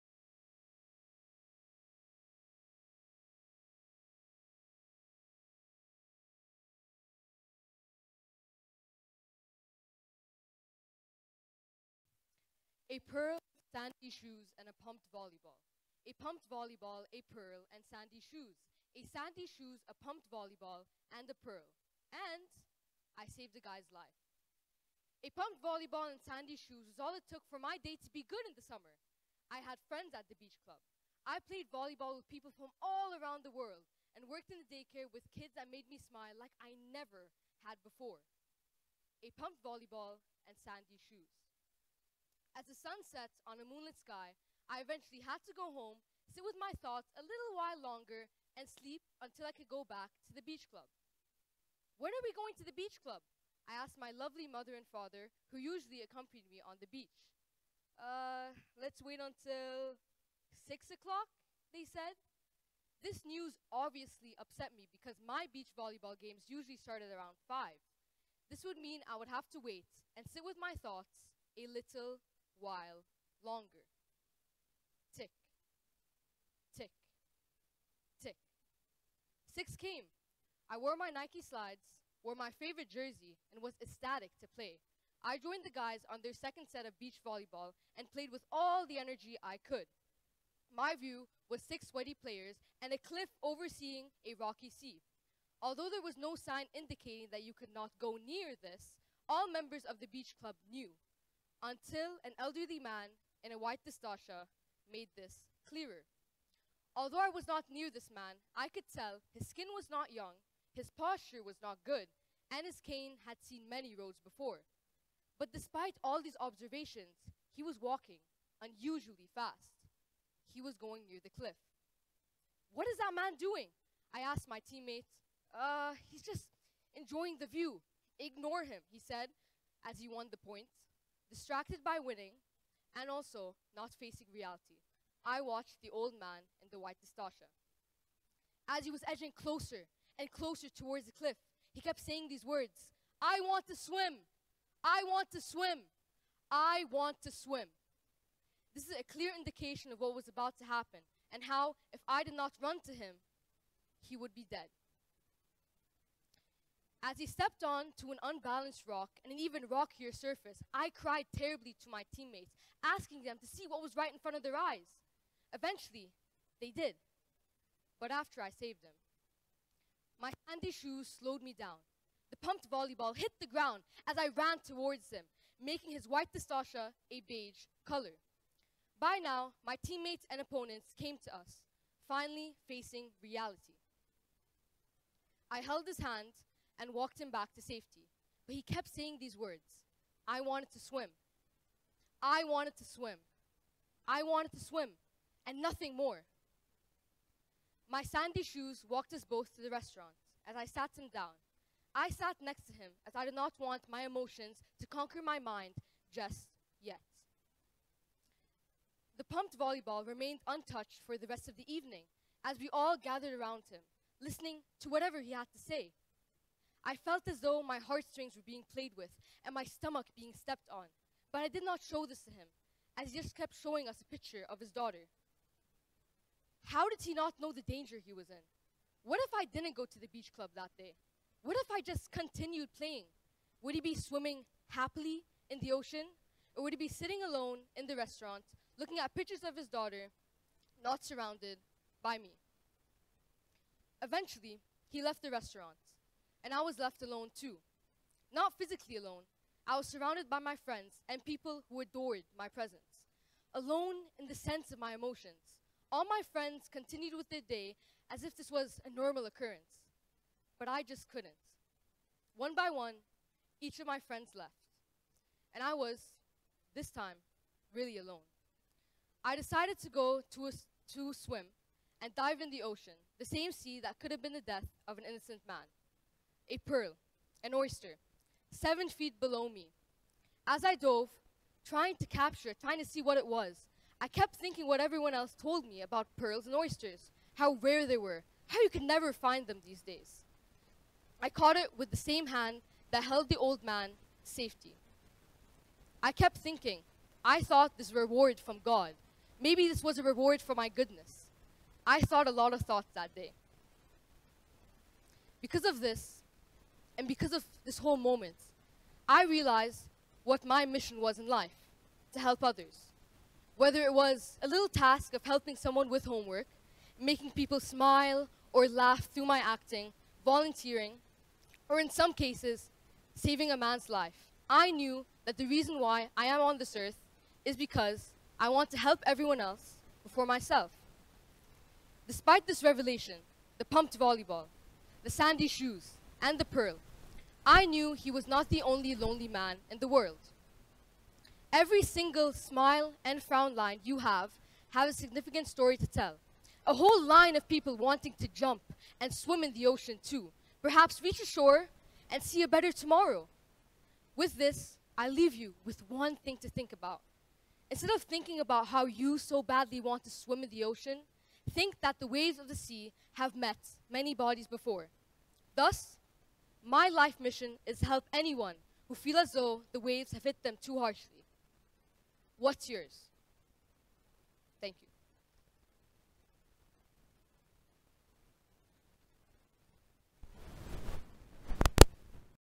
The Pearl | Melanie Abou Saleh | TEDxYouth@ASK
In this touching narrative, the speaker shares a transformative moment that reshaped their outlook on life. Through the simple elements of sandy shoes, a pumped volleyball, and a chance encounter, they unveil a profound lesson about purpose and human connection. Join the speaker on this emotional journey of self-discovery and empathy, and consider the significance of finding purpose in unexpected moments. Melanie Abou Saleh is a junior high honour roll student. She is an actor who also loves to dance and do musical theatre. She is an athlete especially in volleyball, football and badminton, a president and student leader in many different clubs and honour societies such as MUN. Mel loves to volunteer and help out the community in any way too! This talk was given at a TEDx event using the TED conference format but independently organized by a local community.